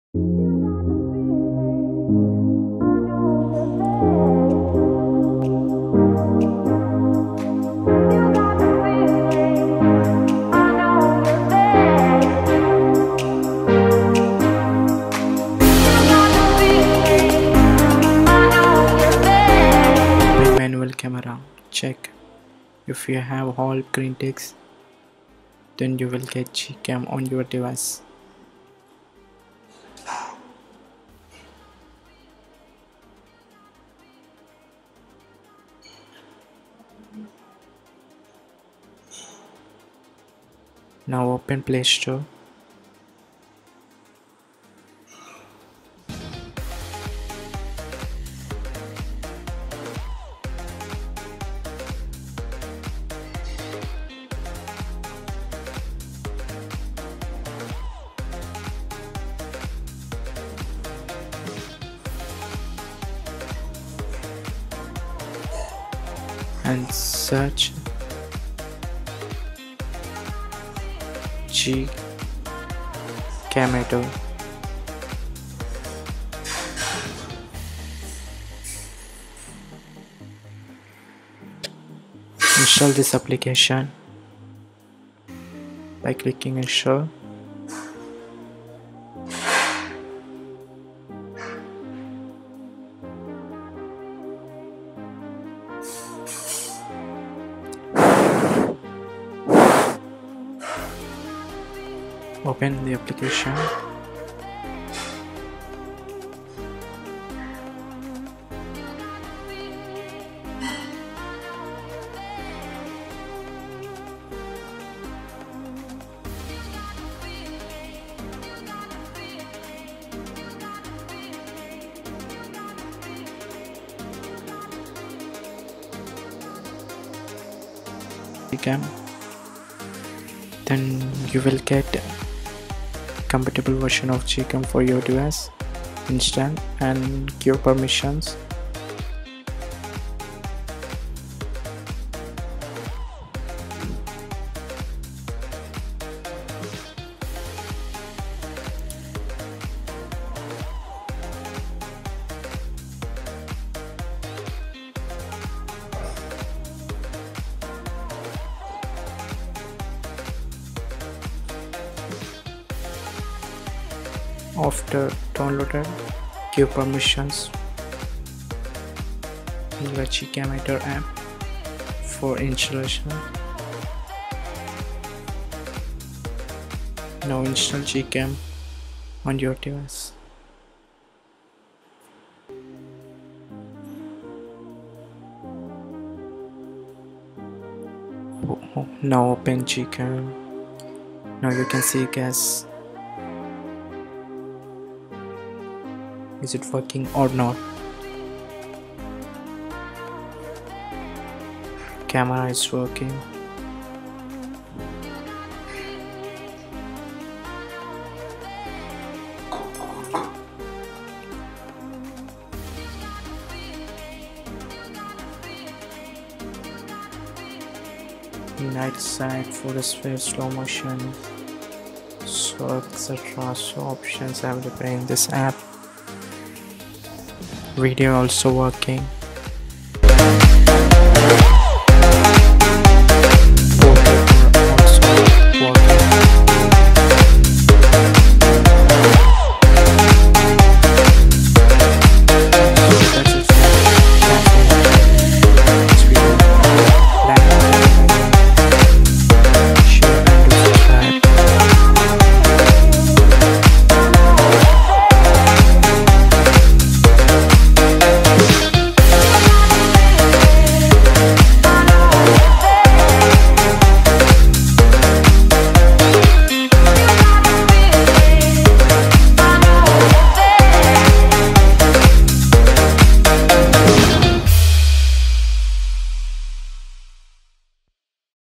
How to Get Perfect Gcam for your Android Device | Install Gcam On Any Android | Redmi 6 Pro (Sakura)
So Hey Guys In This Video I Have Shown How to Install GCam on your Android Device So Before Installation GCam On Your Device You Must Make Sure Your Device Have Enabled Camera2Api option if you have not that. I made a video before watch that video by clicking below link

How to Enable Camera2Api On Redmi 6 Pro (Sakura) | Easy Way To Enable Camera2Api On Any Android 2020